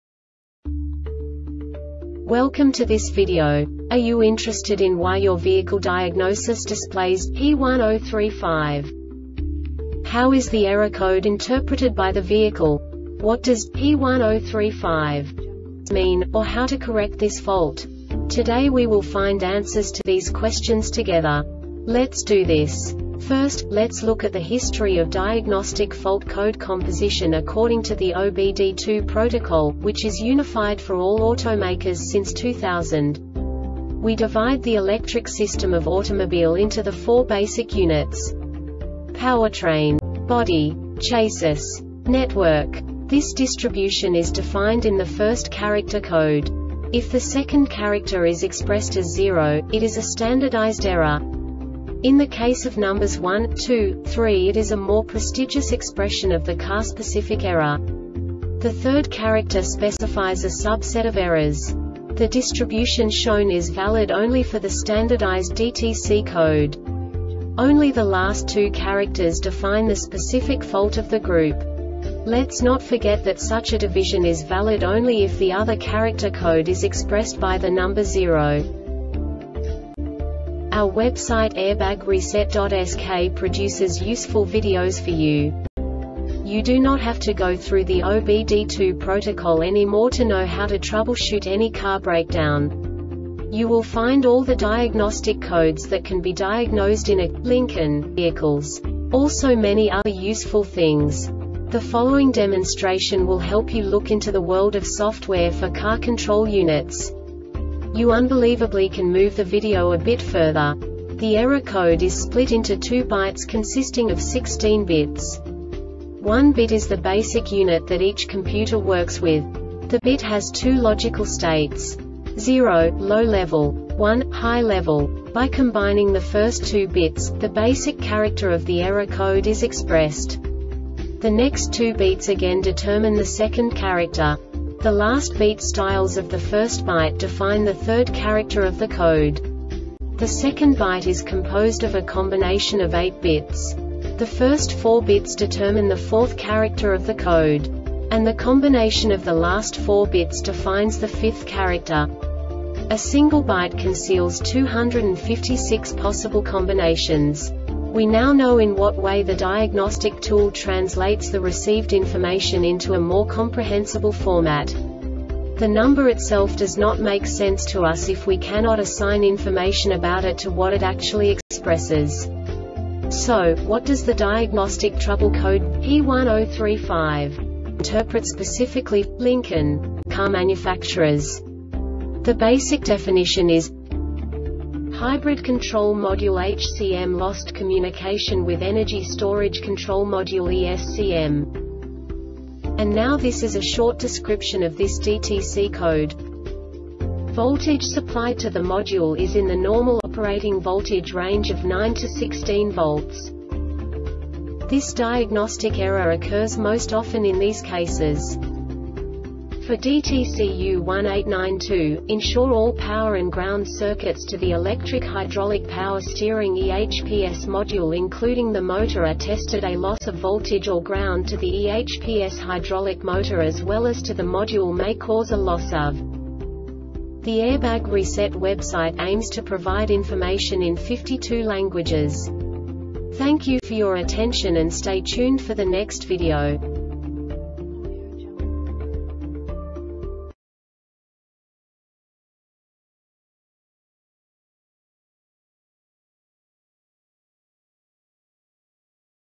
DTC Lincoln P1035 Short Explanation
Contents:
0:21 Basic DTC analysis according to OBD2 protocol standard.
1:48 Insight into programming
2:58 A diagnostically understandable description of the Diagnostic Trouble Code
3:05 Basic error definition

Make
Lincoln
Code
P1035
Definition
Hybrid Control Module HCM Lost Communication With Energy Storage Control Module ESCM
Description